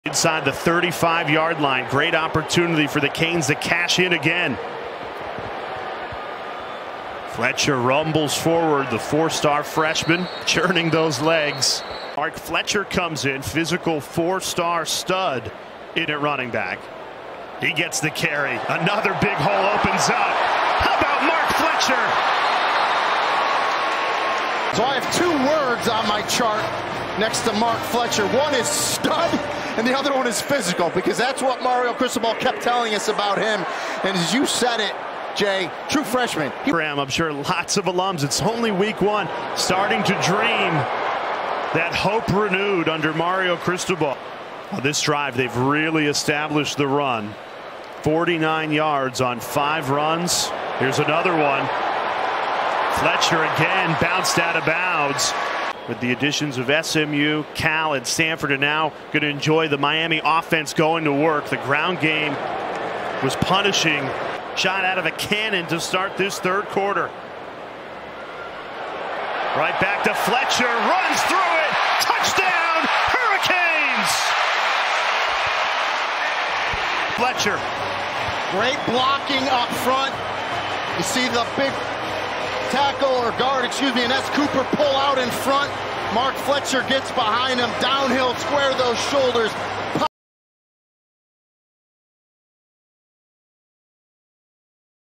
Freshman Mark Fletcher Jr. Excites Miami Fans In Debut
Miami fans have been anxious to see freshman running back Mark Fletcher Jr. play in a Hurricanes uniform, and he did not disappoint in his debut. In his first college game, Fletcher ran for 76 yards and a touchdown on nine carries.